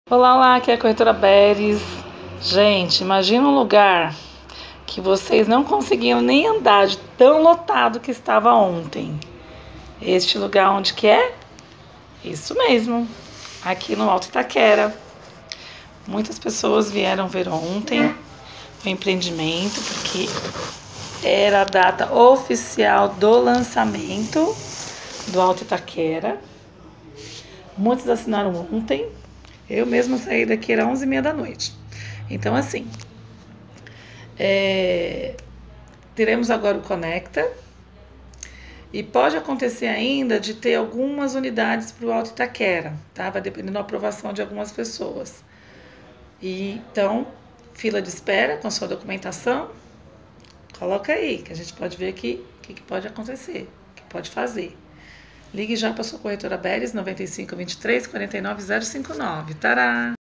1 dia após lançamento Alto Itaquera
Alto Itaquera tem sido um sucesso desde a compra do terreno pela Construtora. Desde Maio estavamos recebendo documentações de clientes e neste último fim de semana 13, 14, 15  foi o lançamento oficial, com muito contratos assinados.
Por conta desse sucesso teremos uma segunda fase com o Conecta Itaquera, que fica a duas quadras da estação Dom Bosco.
Meu nome é Berys. Clique no link ou ligue já: Whatsapp ou número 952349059